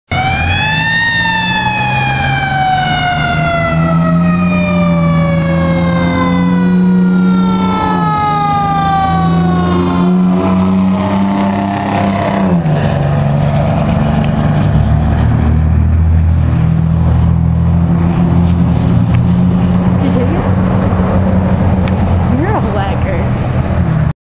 HFD Eng 4 running for Eng 3
Reserve Eng 4 (Temp. fleet Replacement for Eng 3) responded with 4 personnel to a chimney fire on East Irvin Ave in Hagerstown, MD Runing along with E-1, T-1, Utility 3 and Captain 33.